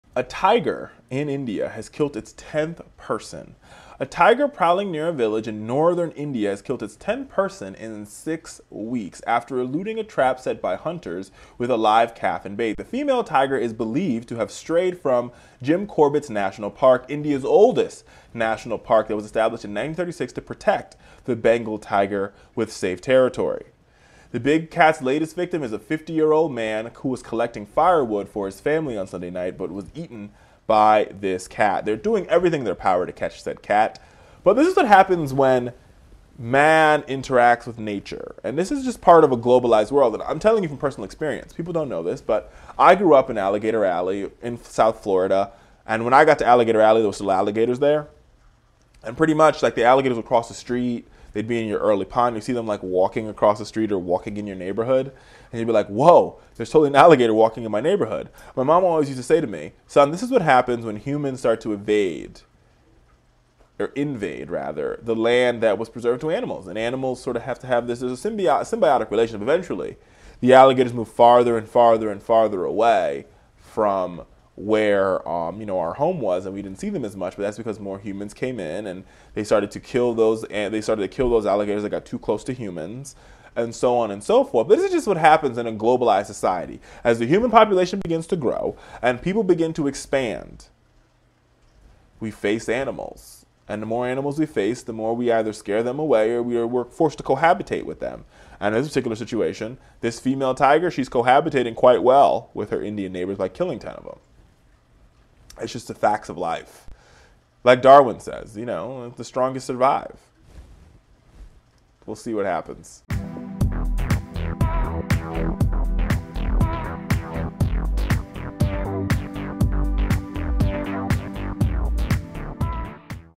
Tiger Evades Hunters, Kills 10th Victim In India
A tiger prowling near villages in northern India killed its 10th person in six weeks, a day after eluding a trap set by hunters with a live calf as bait.

The female tiger is believed to have strayed from Jim Corbett National Park, India's oldest national park, which was established in 1936 to provide endangered Bengal tigers with safe territory.

The big cat's latest victim was a 50-year-old man who was collecting firewood Sunday night in the forest outside Kalgarh village in Uttarakhand state, according to Saket Badola, deputy director of the national park.

The animal ate parts of the man's leg and abdomen before being scared away by villagers waving shovels and metal rods.

Hunters had almost nabbed the tiger a day earlier with a bovine calf.

"On Saturday night the tigress almost fell in trap and was close to the calf," Badola said. "But she did not attack the bait and left silently."

Reports that a killer tiger was on the loose began circulating Dec. 29, when a 65-year-old man was mauled in Sambhal district of Uttar Pradesh state, across the border from Uttarakhand. Since then, thousands of terrified villagers have been told to watch out for the animal and to avoid the forests.

The tiger has been on the prowl across an area spanning some 80 miles (130 kilometers).

"The animal has started attacking humans because it is not getting its natural prey," said Rupek De, chief wildlife warden of Uttar Pradesh. "The tigress must be tired because it is not getting adequate rest."

He said the hunters hired to kill the animal were having trouble tracking it in dense forests. The team also was understaffed; only three of the six hunters hired for the job showed up for work, De said.

De said he asked wildlife officials in Uttarakhand for help, saying there seems to be lack of co-ordination.

On Sunday, angry villagers seized a national forestry office, demanding protection and compensation for the families of the dead.

"We can understand the predicament of the villagers," Badola said. "The villagers do not have toilets in their homes. They go out in the open or forest areas to answer nature's call. In this scenario it is difficult to give protection to each and every villager. We have advised them to move in groups."

India's wild tigers are considered endangered because of rampant poaching and shrinking habitat as India undergoes breakneck development to accommodate the staggering growth of its 1.2 billion people.

India today has more than half of the 3,200 tigers estimated to be left in the wild. Despite dozens of tiger reserves across the country, however, the numbers have sunk from an estimated 5,000-7,000 in the 1990s, when the big cats' habitat was twice as large.
3 Steps To Join FowlerNation!
2. 'Like' The Richard Fowler Show on Facebook
3. 'Follow' Us on Twitter